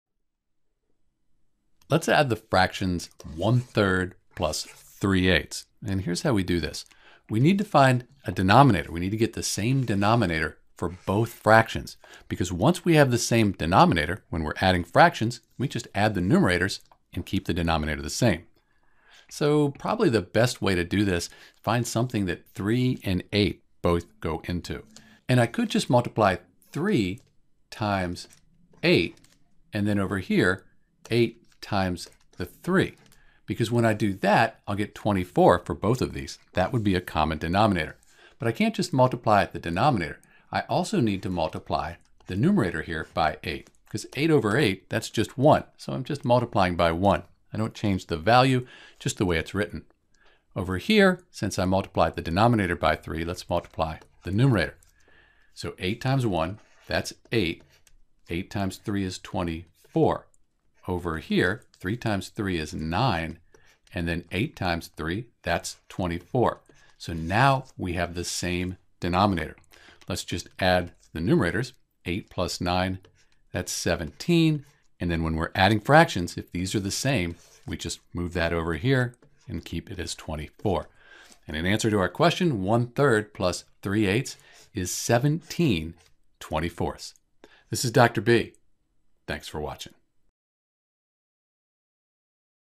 How to Add 1/3 + 3/8
Note that the lowest common multiple (LCM) is also called the lowest common denominator (LCD). You can find the LCM for and by listing all the multiples for 3 and 8. Then you select the lowest number that is the same for both numbers.

A quick way to check your work is provided at the end of the video. When you add 1/3 + 3/8 you should get the same fraction for both methods.